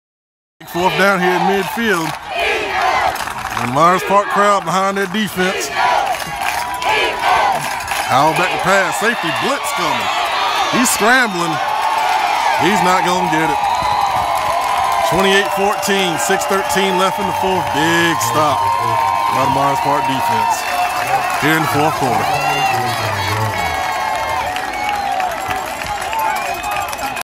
BIG Stop By Myers Park Defense 4th Quarter
Myers Park at Sun Valley Week 5 2016 Season